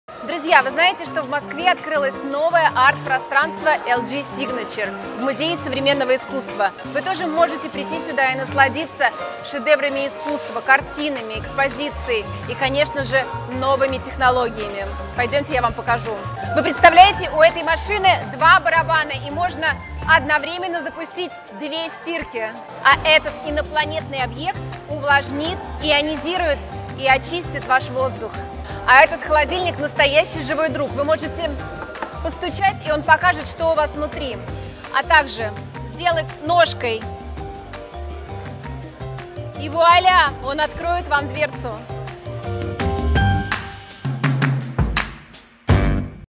Торжественный вечер в ММОМА. Часть 8. Анастасия Гребёнкина
В рамках "Российской недели искусства LG SIGNATURE в ММОМА" в особняке Московского музея современного искусства на Петровке 25 прошел торжественный вечер, организованный совместно с журналом TATLER. Его открыли амбассадоры бренда LG SIGNATURE телеведущая Екатерина Одинцова и танцор и хореограф Евгений Папунаишвили, после чего гости приветствовали доктора экономических и психологических наук, колумниста TATLER Алексея Ситникова. LG выражает огромную благодарность главному тренеру сборной России по футболу Станиславу Черчесову за посещение этого культурного события. На сцене к собравшимся обращались послы LG SIGNATURE Сати Спивакова, Галина Юдашкина и Уиллиам Ламберти. Гостями стали представители российского бизнеса и искусства, а также послы и друзья бренда LG SIGNATURE и ММОМА: Юлия Барановская, Анастасия Гребенкина, Лариса Вербицкая и многие другие. Красивым завершением программы стал танец солистов труппы Большого театра — Анны Тихомировой и Артёма Овчаренко. Посмотрите, как это было.